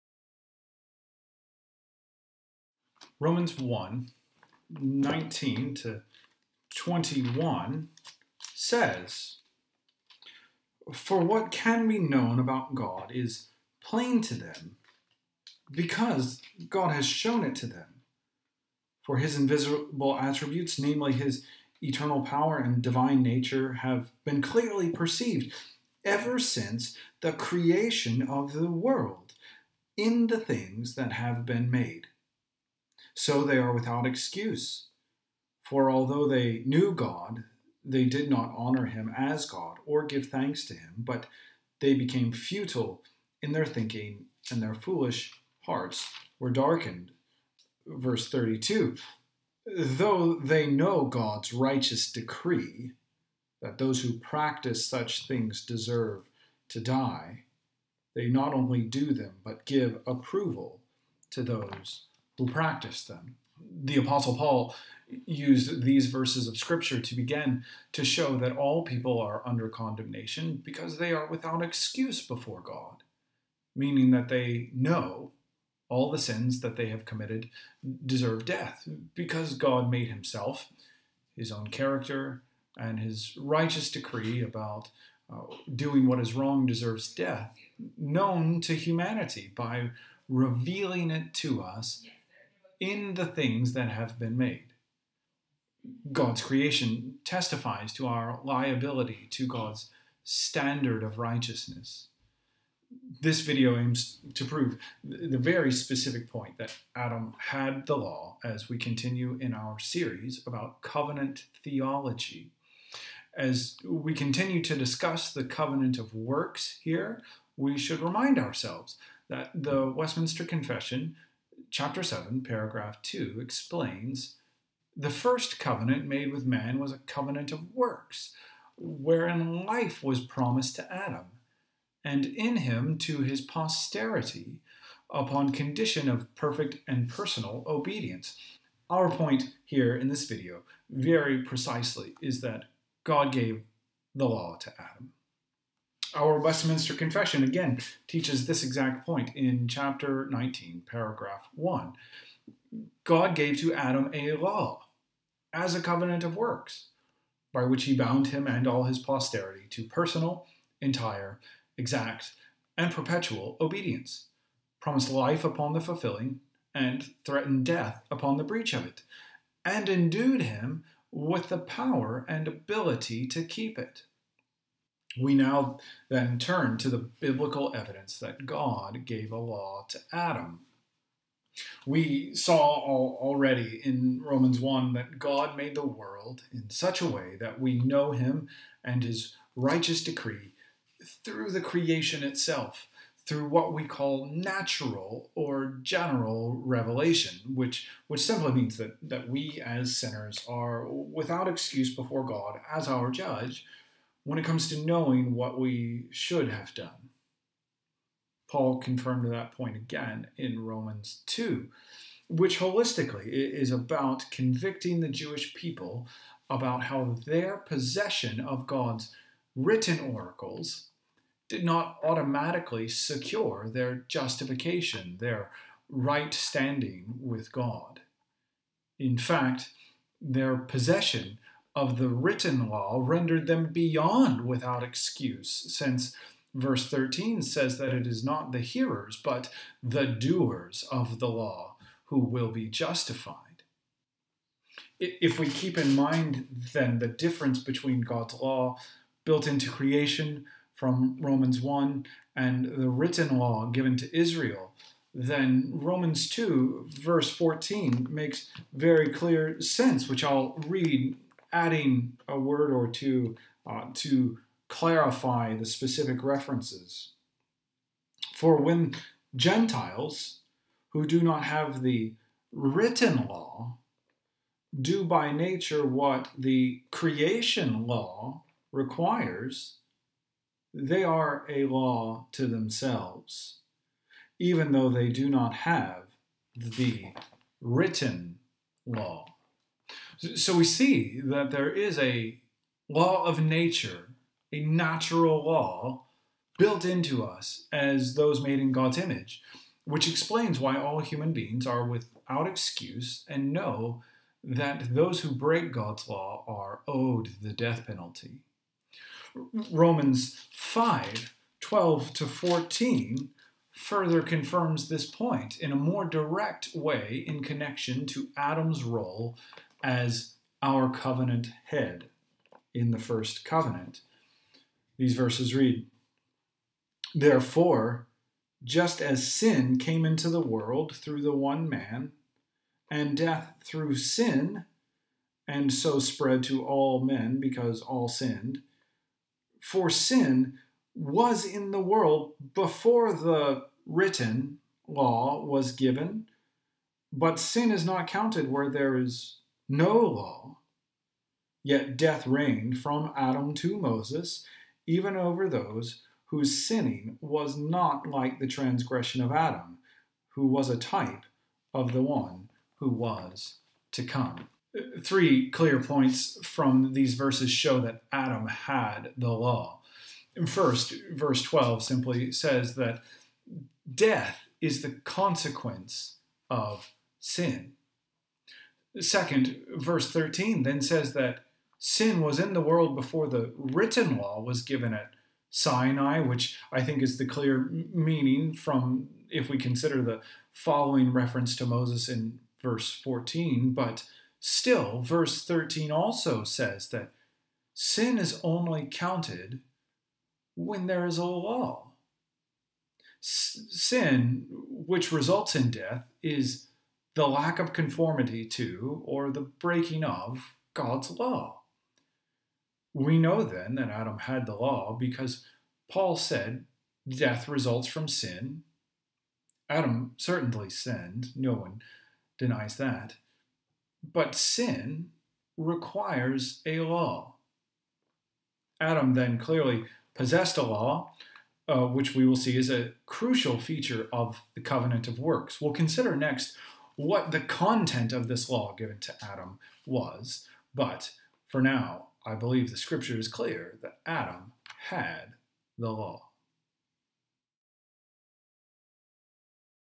(5) The Covenant of Works B) Adam Had The Law
Rev Dr Harrison Perkins continues his series on Covenant Theology with this look at the Covenant of Works.